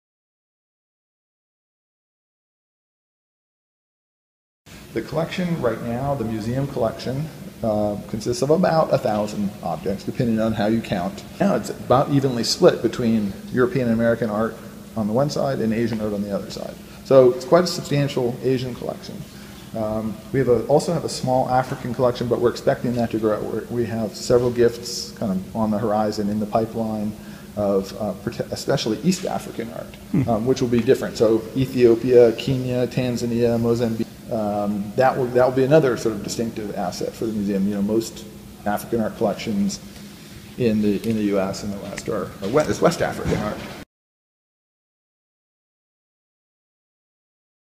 Hope College's Kruizenga Art Museum
Hope College's Kruizenga Art Museum debuts with an exhibition of work titled "Past Present East West" from the college's permanent collection.